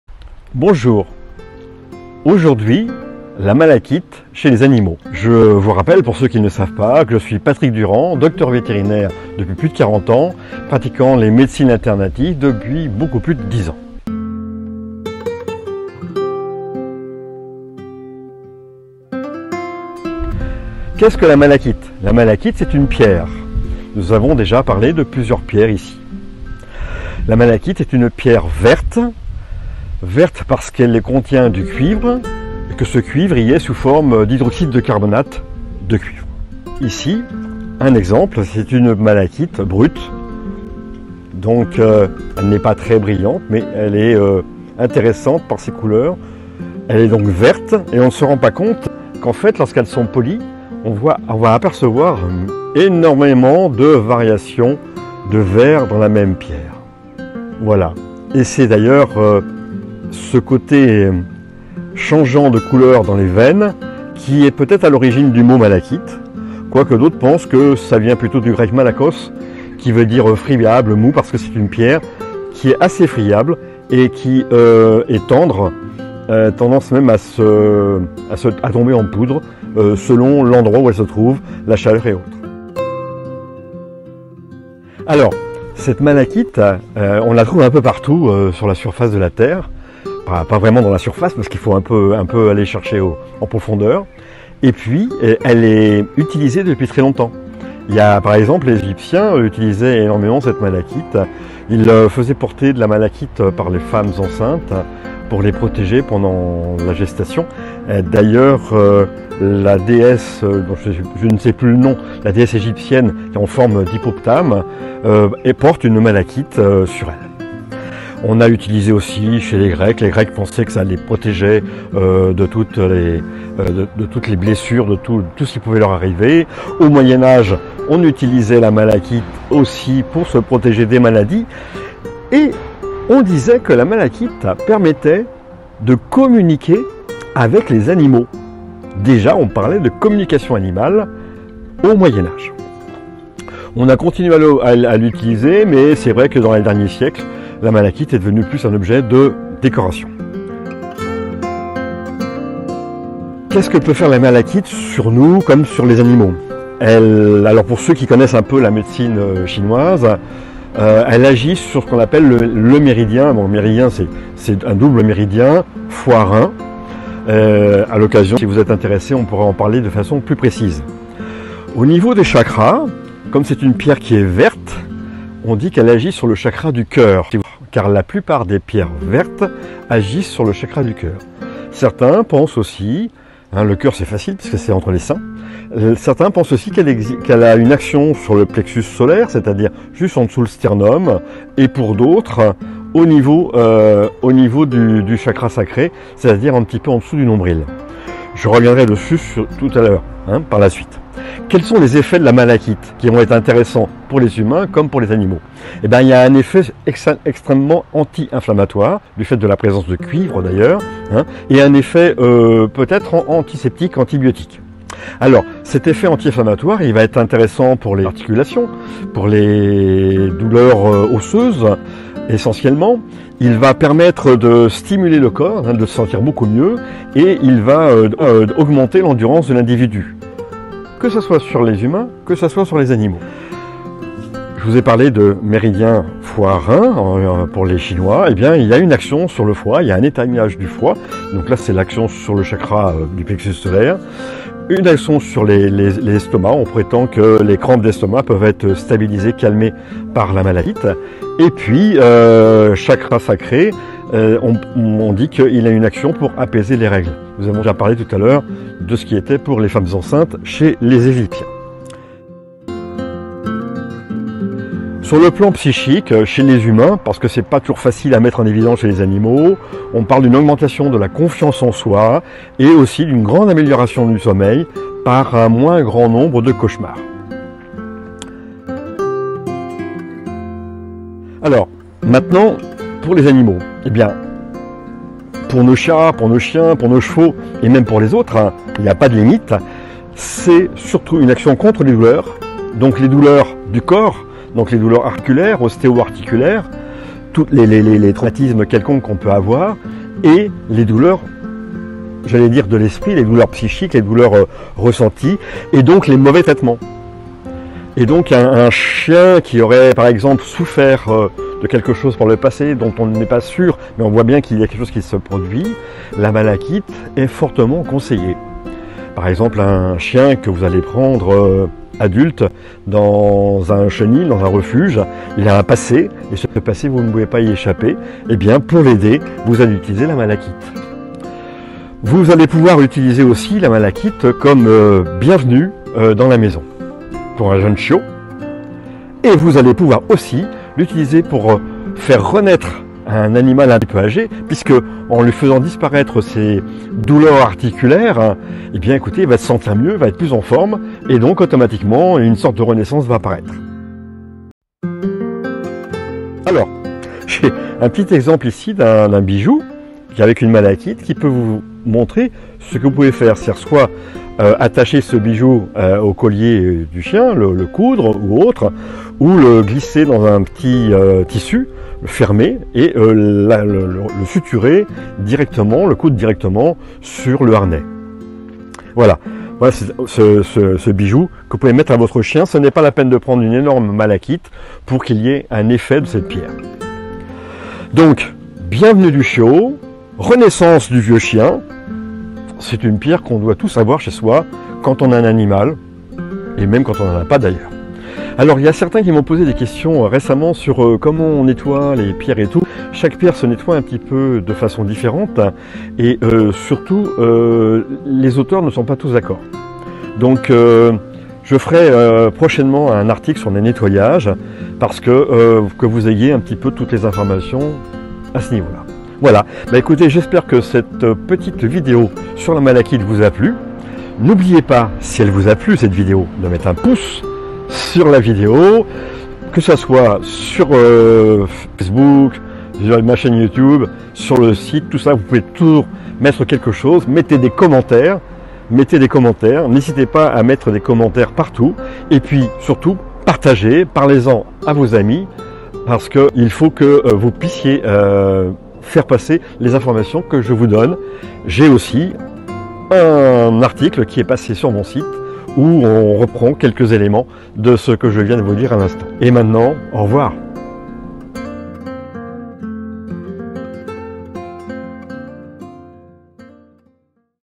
La malachite en naturopathie, la pierre à toujours avoir chez soi
Une pierre verte, la Malachite, est très utilisée pour ses propriétés apaisantes dans les cas de douleurs ostéo-articulaires chez les humains. Ceci est certainement lié à la présence de cuivre et c'est également le cas chez les animaux.

Elle est utilisée depuis des millénaires en commençant par les Égyptiens de l'Antiquité et au Moyen-Âge, l'on pensait qu'elle permettait de communiquer avec les animaux, de quoi en faire une pierre très intéressante en Naturopathie Animale.

Pour les animaux, cette pierre est un puissant allié suite à des mauvais traitements de toute sorte, pour lutter contre les problèmes de douleurs de type arthrite, arthrose.

Au travers de cette vidéo, le Dr Patrick Durand, Naturopathe Animalier et Dr Vétérinaire vous en dit plus sur cette pierre incontournable de la lithothérapie.
Pour aller plus loin, n'hésitez pas à aller consulter son article sur le sujet

Youtube : Patrick DURAND - Naturopathe Animalier